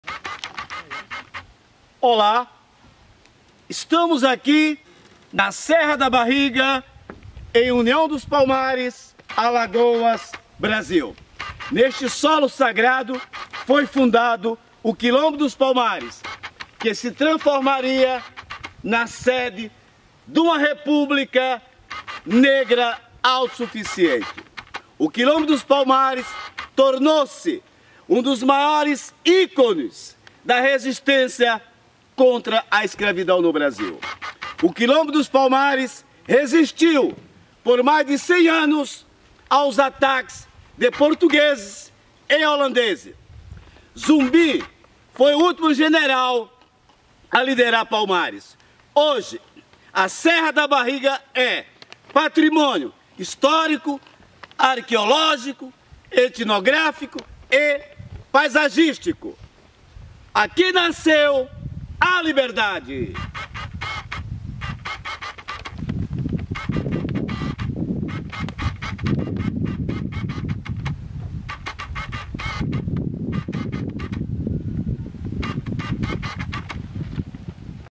Prof. Ismael Dantas e sua equipe visitam o Parque Memorial Quilombo dos Palmares.
Prof. Ismael Dantas e sua equipe (Ana Paula e Ismaelito) visitam o Parque Memorial Quilombo dos Palmares. Em União dos Palmares no estado de Alagoas, Brasil